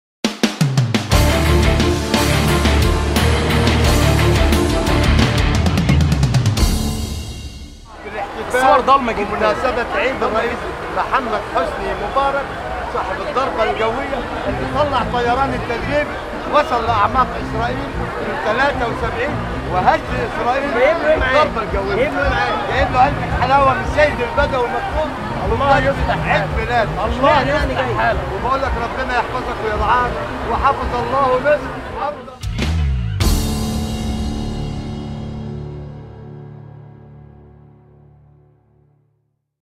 "شبيه السادات" في عيد ميلاد مبارك: "جايبله علبة حلاوة مخصوص"
تقرير:  بيتر حكيم

جريدة الوطن
جريدة يومية مستقلة